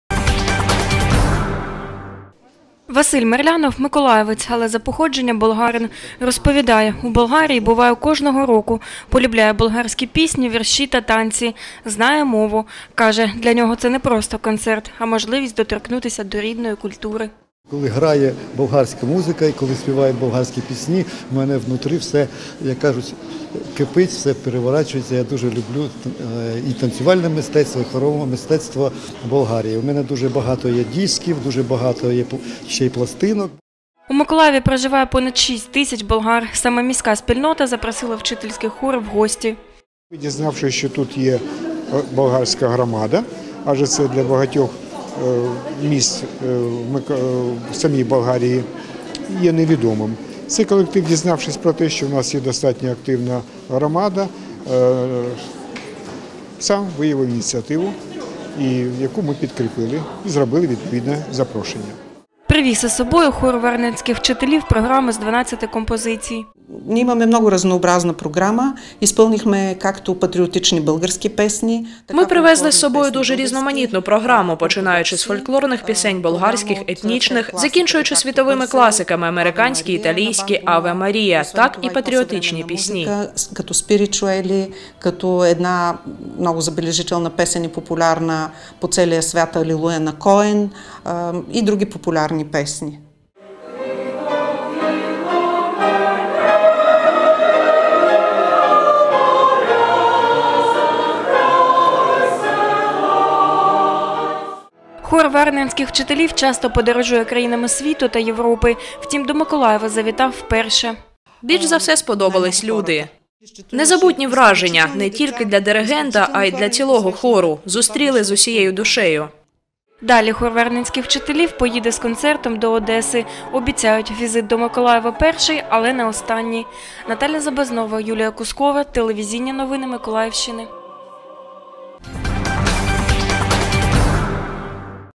Хор варненських вчителів “Арс Музика” завітав до Миколаєва
Із нагоди встановлення 100-річчя дипломатичних відносин між Україною та Болгарією, до Миколаєва завітав хор варненських вчителів “Арс Музика”.
Яку програму підготували для містян — дивіться далі.